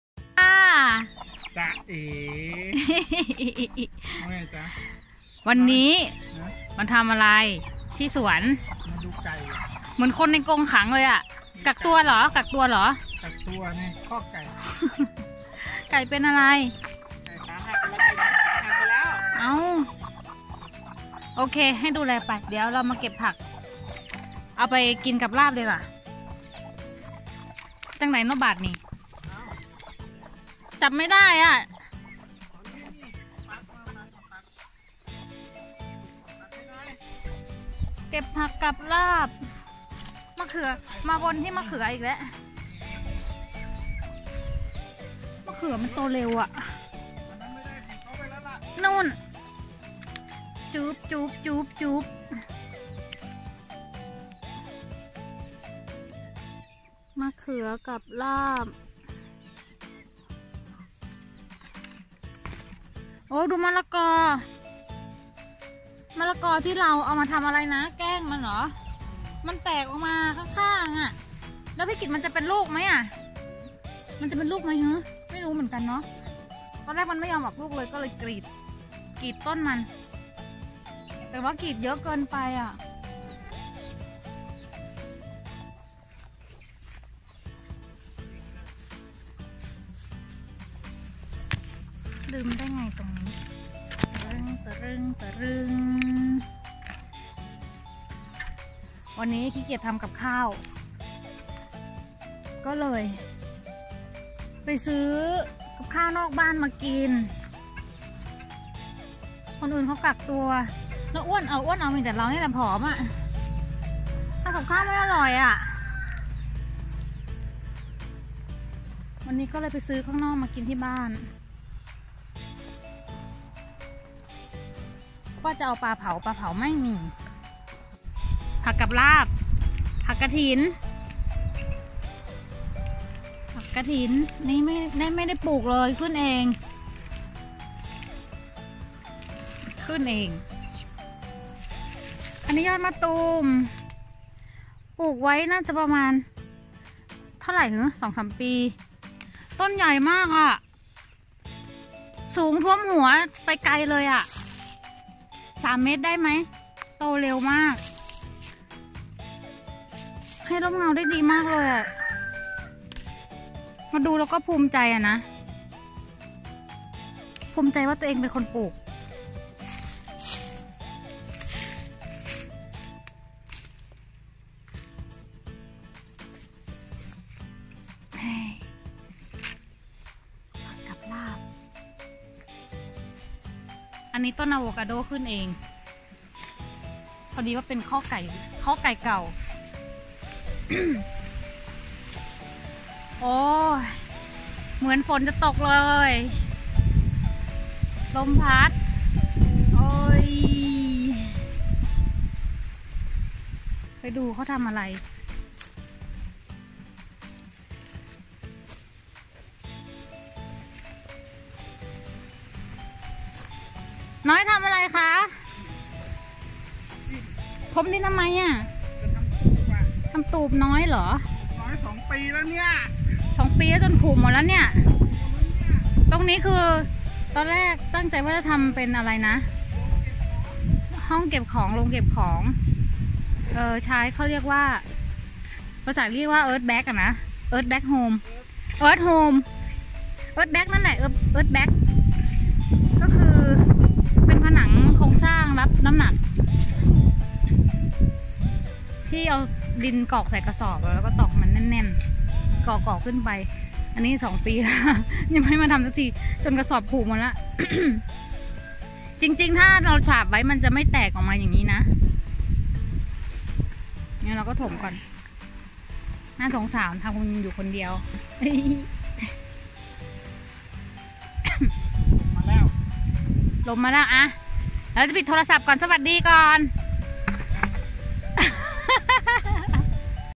ผู้จัดการมาดูกิจการ​ ชมสวนน้อยในยามวิกฤต​ โควิด19
ช่วงนี้แดดร้อนคุณภรรยาก็จะไม่ค่อยจะอยากมาเดินที่สวนเท่าไหร่​ เพราะมันร้อนจริงๆ​ ส่วนมากจะใช้ผมให้มาเก็บผักเสียมากกว่า​ แต่วันนี้เขาลงมาเดินเล่นที่สวนช่วงเย็นๆพร้อมกับจะพาชมสวนเองว่างั้นไปดูแหล่งอาหารของเรากันครับ